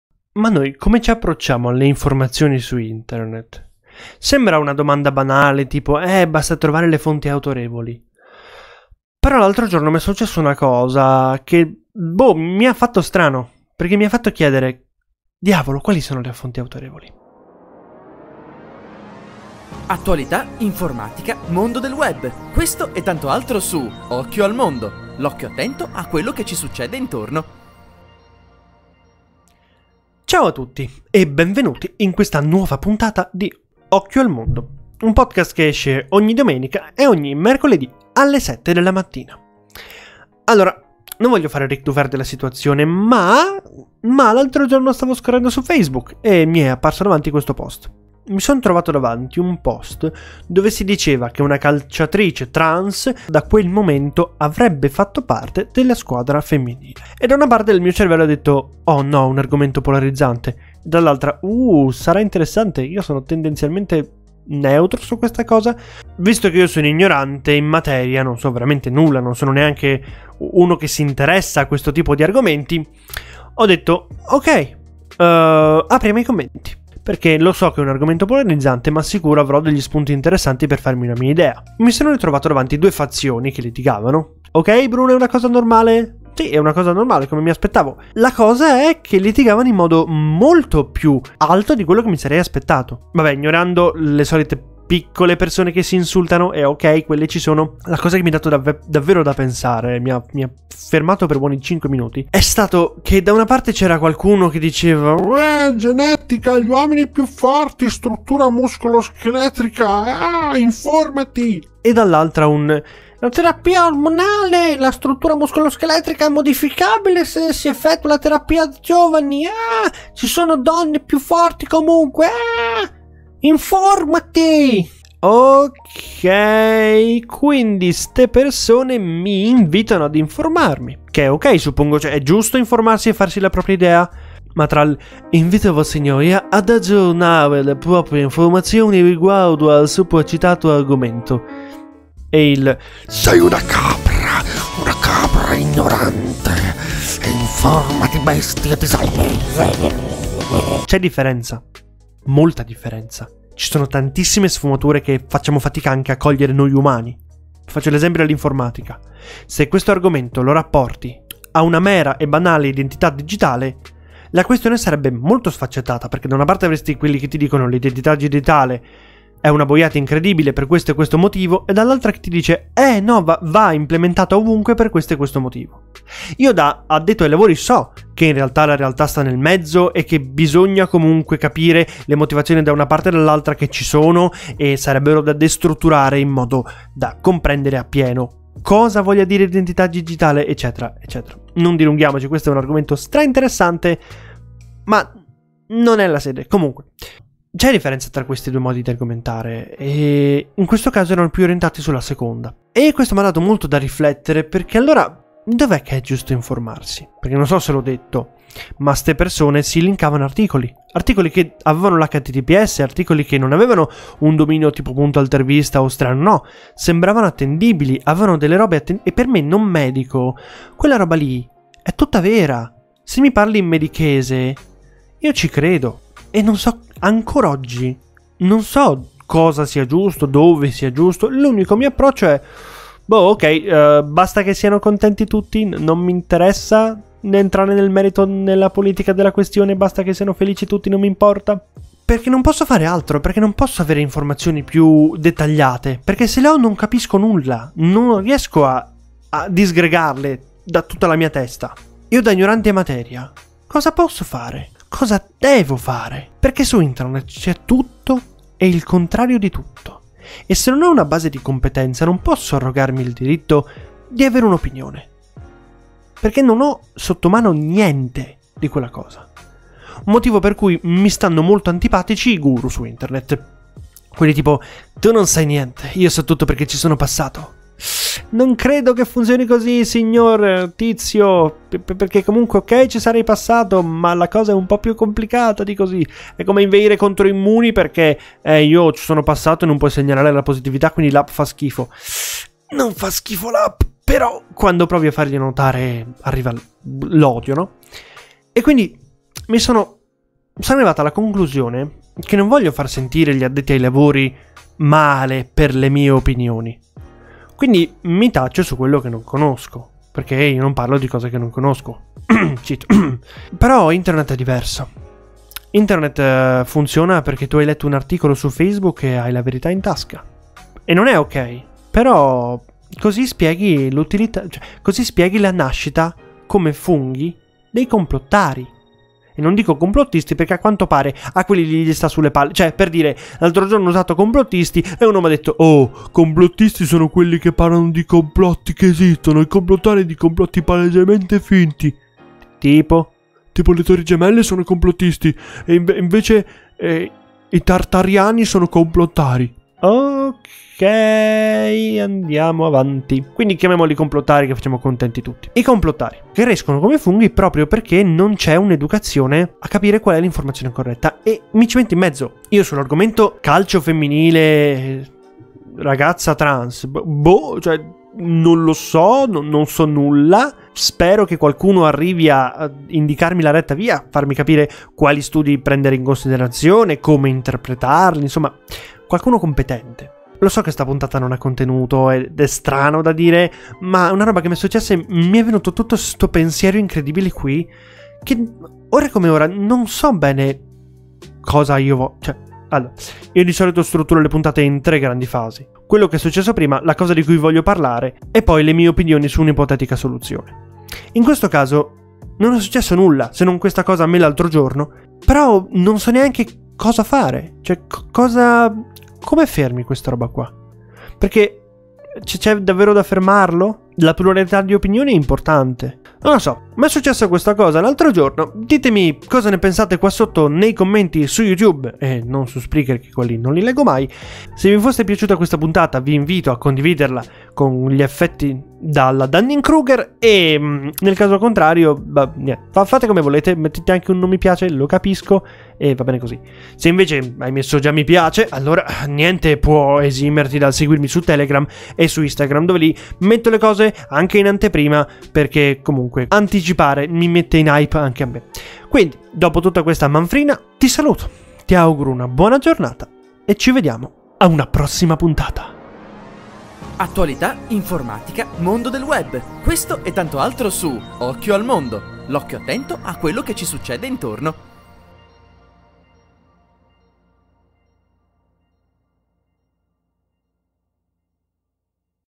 Informazioni su internet? Non sto capendo nulla
L'altra sera mi sono trovato davanti un post dove si diceva che una calciatrice trans da quel momento avrebbe fatto parte della squadra femminile... e ho aperto i commenti... Forse non avrei dovuto